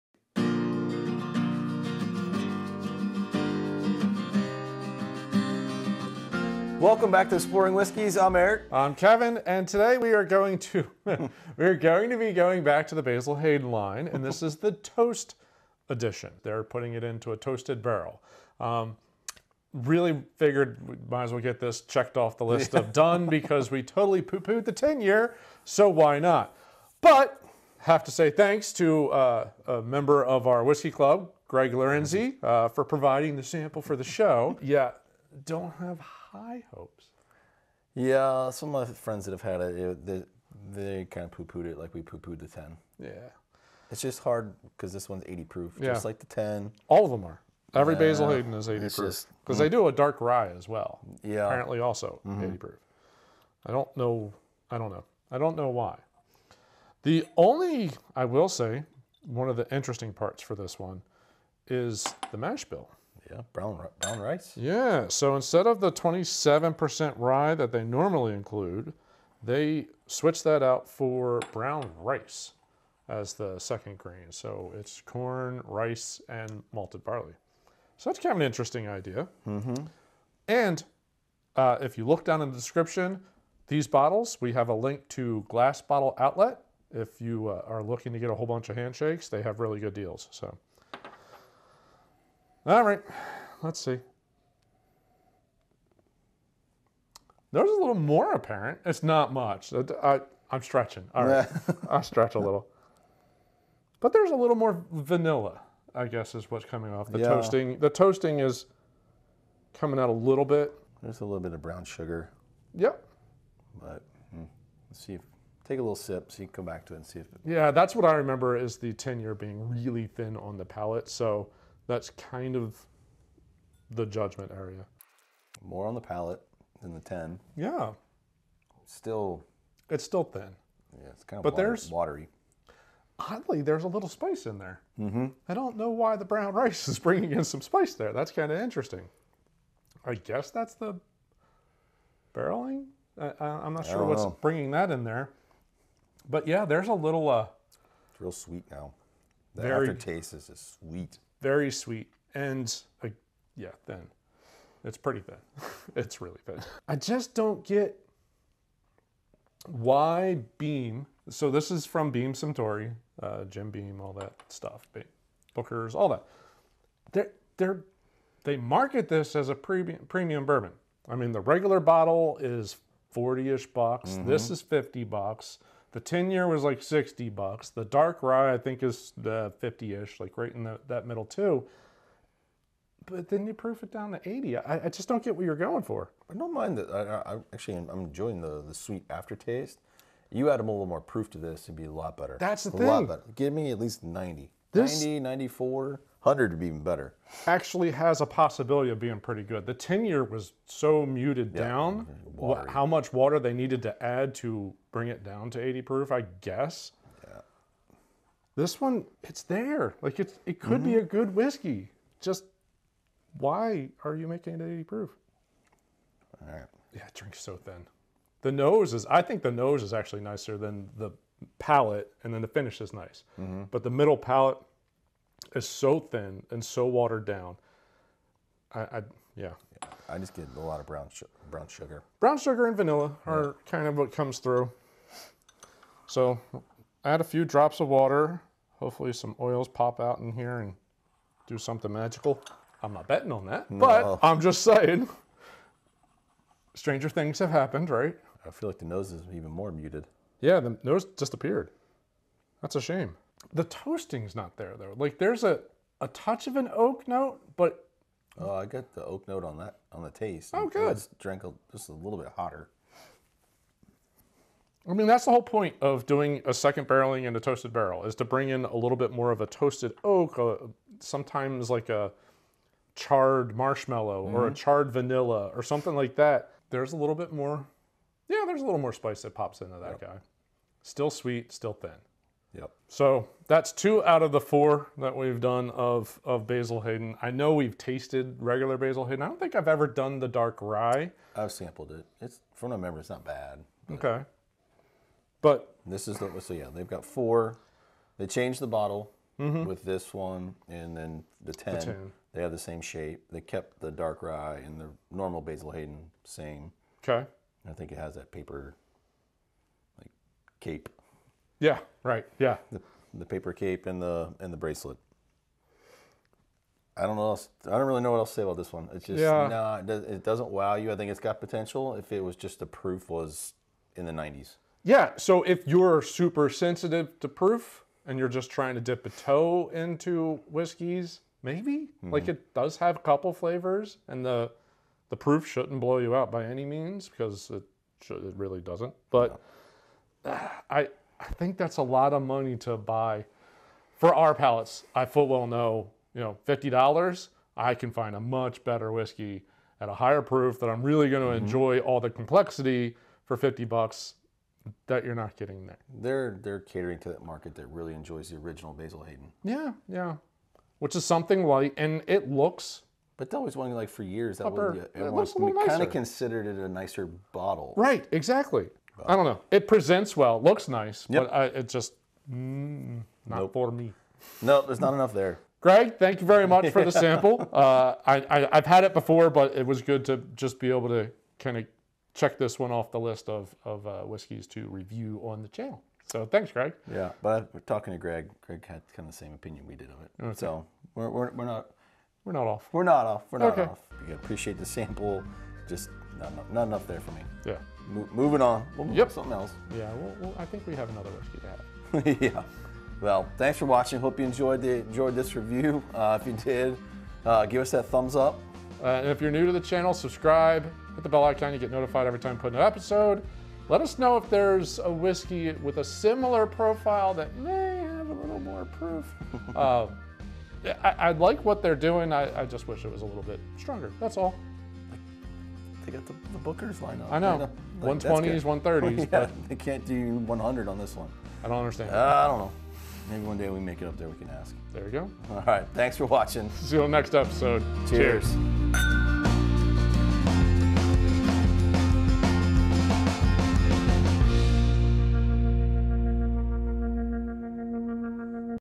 Basil Hayden Toast Finished Bourbon Review...AGAIN WHY PROOF DOWN TO 80???
Today we sample a toasted bourbon from Jim Beam...Basil Hayden Toast!!! This is the fourth permanent edition in the Basil Hayden’s brand line. This has a different mashbill for the brand...replacing the usual high rye of 27% rye with 27% brown rice. Toast also undergoes a secondary toasted barrel finish for an undisclosed amount of time before being proofed down to 80.

The nose is again too light but there a few notes you can get from the toasted barrel, like vanilla and brown sugar. The palate has a little more body than the 10 year but it's still thin...there is a touch of spice that comes through which is odd since there isn't any rye in the mashbill. The finish is sweet but thin and short. With a small amount of water, the nose total mutes out...very disappointing. The palate adds a little more spice heat coming through but really just flat overall. While better and cheaper than the 10 year, we don't recommend this...there are better bottles out there. We will keep an eye out to see if they change the proof in future releases! Cheers!!!

Thanks for the sample Greg!!

If you have a whisk(e)y you would like us to review, leave a note in the comments!!!

Timestamps:
00:00 Intro
00:11 Whisky details & background
01:54  Nose & palate conversation - Neat
03:55  Nose & palate conversation - Water added
06:34 Outro